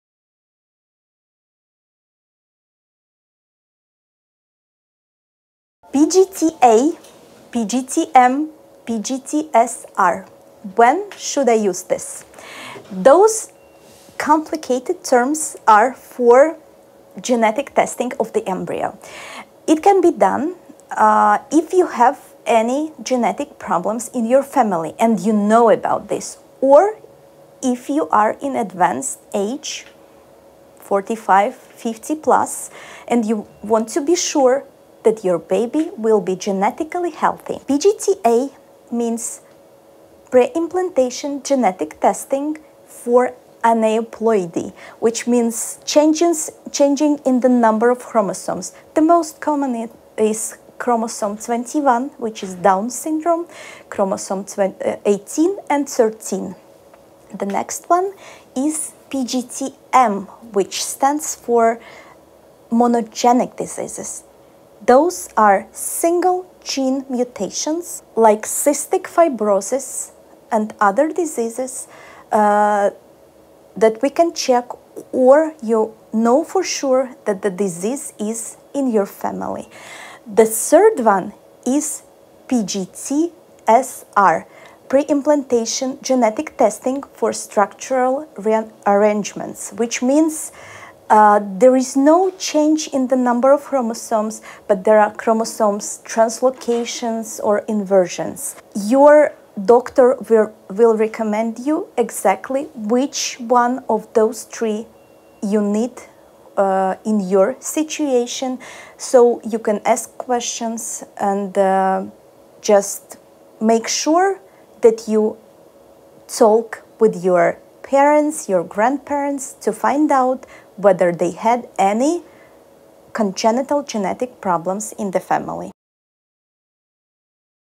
What is the difference between PGT-A / PGS and PGD?
"PGT-A, PGT-M, PGT-SR, those complicated terms are for the genetic testing of the embryo. It should be done if you have any genetic problems in your family, and you are aware of it, or if you are in advanced age 45-50 plus, and you want to be sure that your baby is genetically healthy", Dr Oksana Babula,
Gynaecologist, Fertility Specialist at EGV Clinic explained.